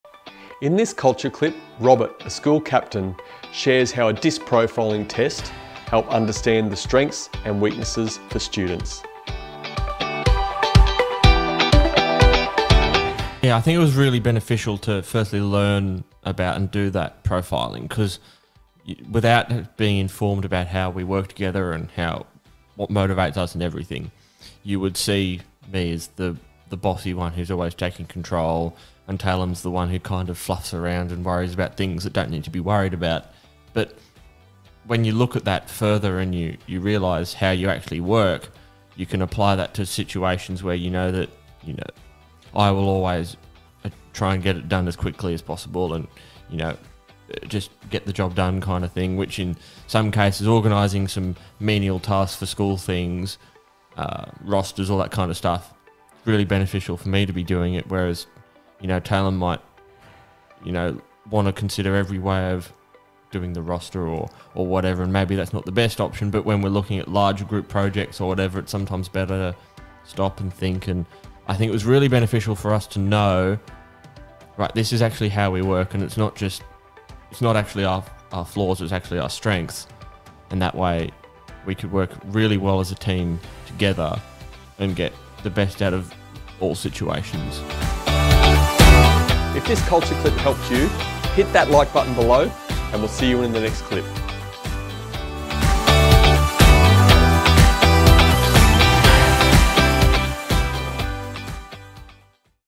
Strengths and Weaknesses for Students (DiSC Profiling for School Captains)
How would you identify strengths and weaknesses for students? Using a DiSC profiling tool, we helped two school captains understand their own and each others strengths and weaknesses, allowing them to work better together and ultimately be better school leaders.

0:00 Introduction
0:12 Strengths and weaknesses for students (DiSC profiling for school captains)
1:28 End card & Subscribe